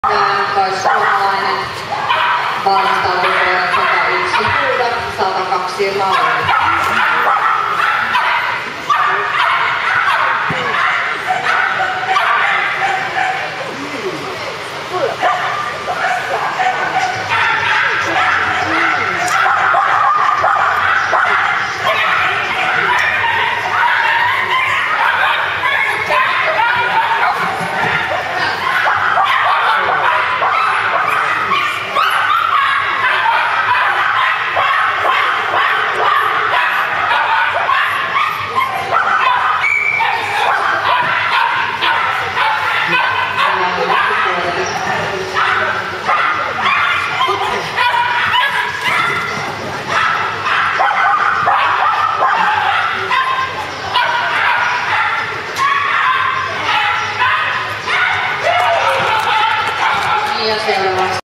Cooper 20170304 jumpC
Agility sheltie Cooper on mini3 jump course in Sipoo on the 4.3.2017. Result DIS.